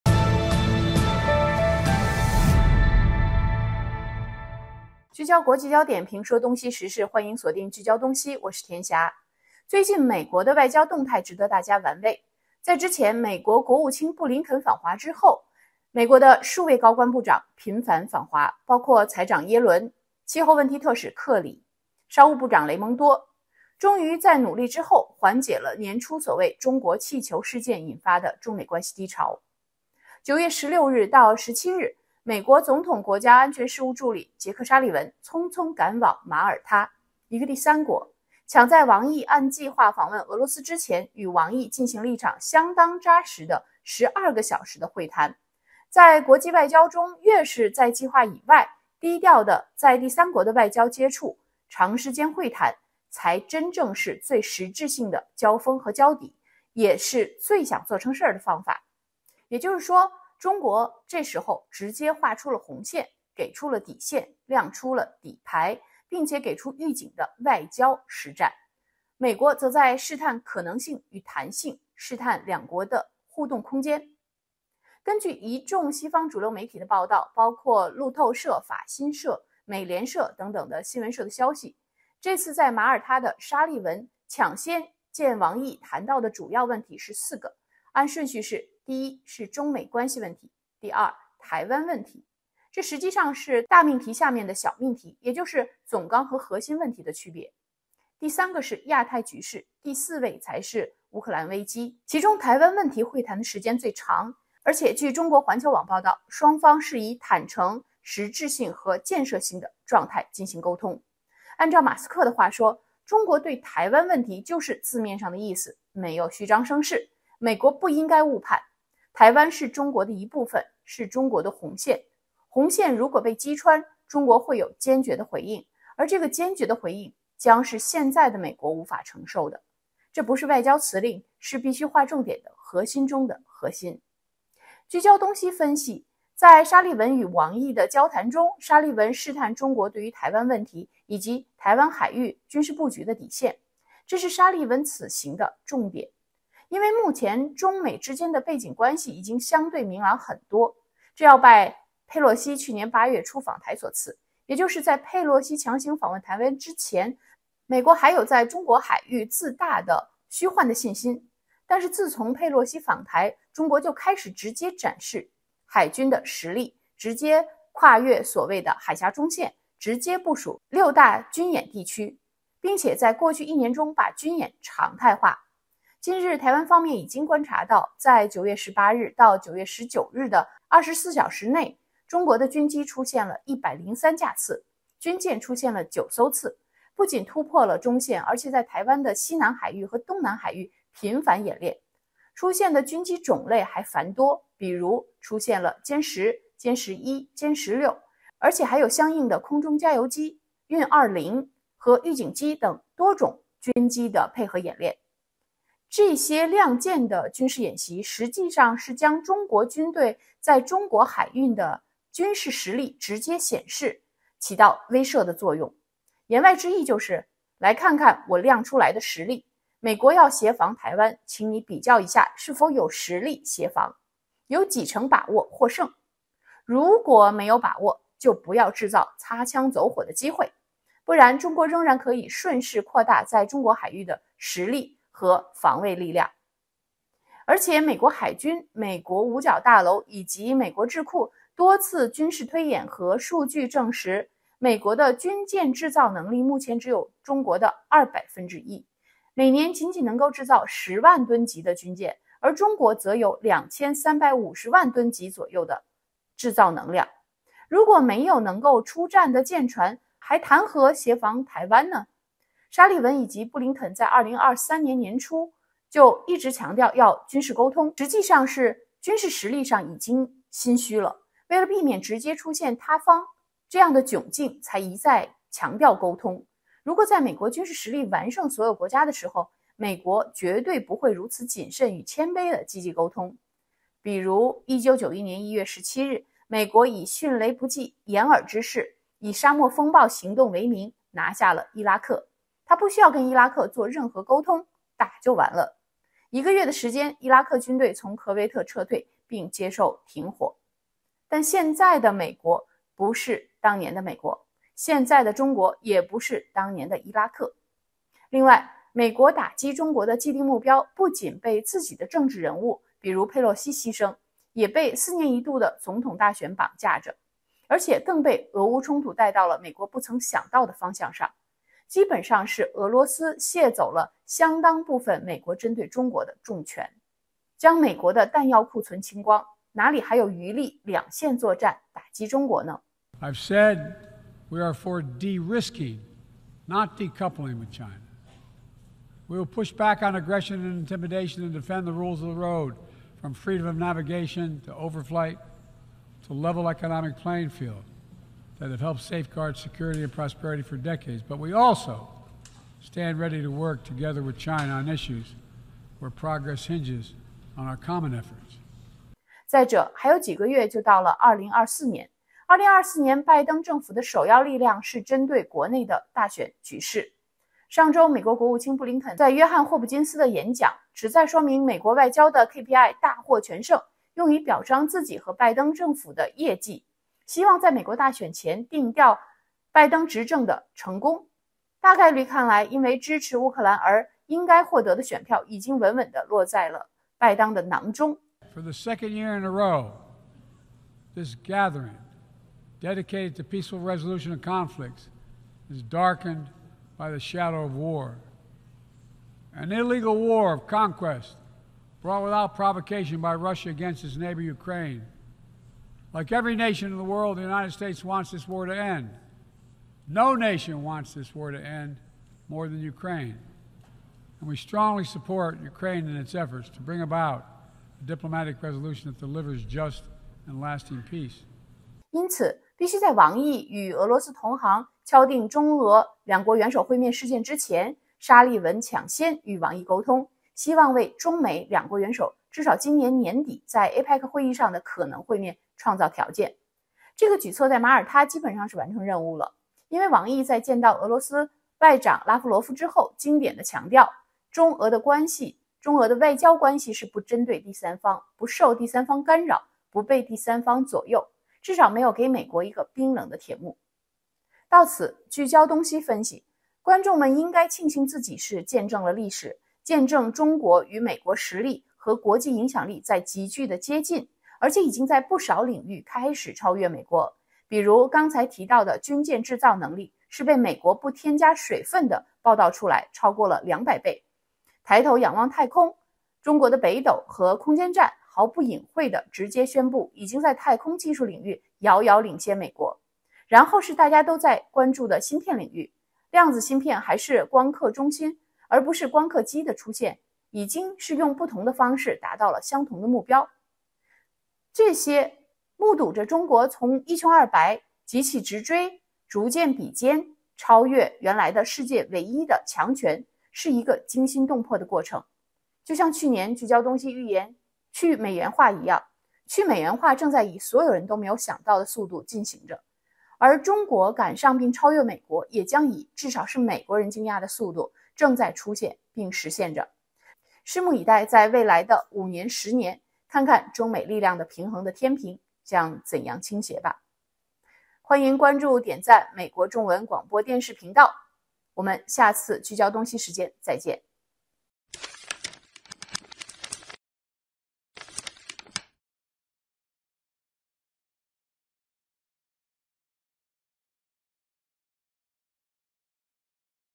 沙利文突然密会王毅 掩饰不住的焦虑？【聚焦东西】美国
00:00 聚焦东西
00:05 美国高官密集访华布林肯、耶伦、克里、雷蒙多；
00:30 沙利文马耳他突然密会王毅；
01:05 中国对台湾问题亮出红线；
02:31 美国外交失去焦点被大选绑架、被俄罗斯卸力；
02:55 今日中国军方创纪录派遣103架军机与9艘军舰在台湾海域军演；
03:56 美军方承认中方造舰能力是美国的200倍；
04:43 沙漠风暴行动直击伊拉克；
05:33 美国不谋求与中国脱钩；
06:09 布林肯在约翰霍普金斯大学演讲；
07:35 见证历史中国正快速逼近美国并在部分领域超越美国；